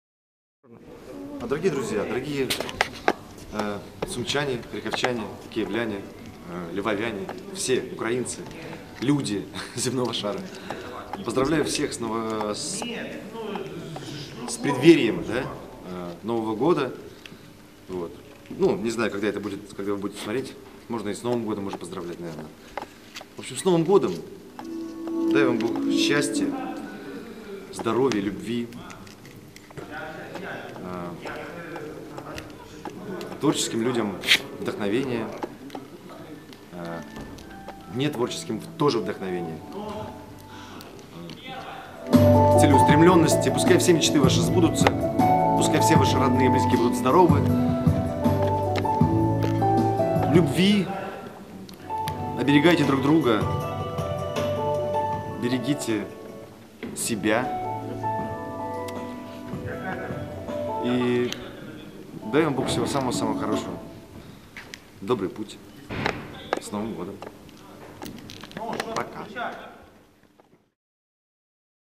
Сергей Бабкин Новогоднее поздравление Сумы 2012
Сергей Бабкин Новогоднее поздравление Сумы 15 декабря 2012